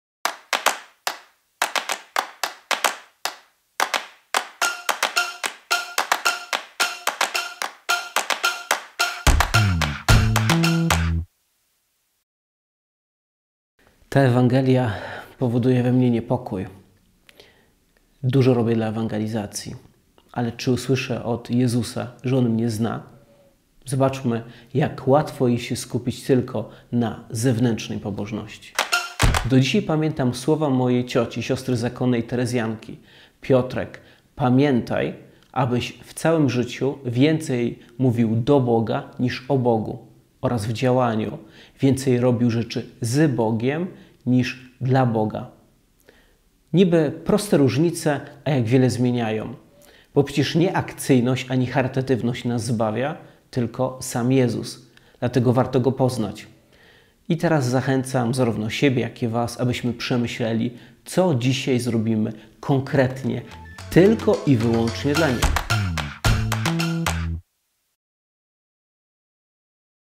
[JM#34] Czy Jezus się do mnie przyzna?
Przeczytaj fragment Ewangelii Łk 13, 22-30:
Jezus przemierzał miasta i wsie, nauczając i odbywając swą podróż do Jerozolimy. Raz ktoś Go zapytał: «Panie, czy tylko nieliczni będą zbawieni?»
On rzekł do nich: «Usiłujcie wejść przez ciasne drzwi; gdyż wielu, powiadam wam, będzie chciało wejść, a nie zdołają.
Skoro Pan domu wstanie i drzwi zamknie, wówczas, stojąc na dworze, zaczniecie kołatać do drzwi i wołać: „Panie, otwórz nam!”, lecz On wam odpowie: „Nie wiem, skąd jesteście”. Wtedy zaczniecie mówić: „Przecież jadaliśmy i piliśmy z Tobą, i na ulicach naszych nauczałeś”.
Lecz On rzecze: „Powiadam wam, nie wiem, skąd jesteście. Odstąpcie ode Mnie wszyscy, którzy dopuszczacie się niesprawiedliwości!” Tam będzie płacz i zgrzytanie zębów, gdy ujrzycie Abrahama, Izaaka i Jakuba, i wszystkich proroków w królestwie Bożym, a siebie samych precz wyrzuconych.
Przyjdą ze wschodu i zachodu, z północy i południa i siądą za stołem w królestwie Bożym. Tak oto są ostatni, którzy będą pierwszymi, i są pierwsi, którzy będą ostatnimi».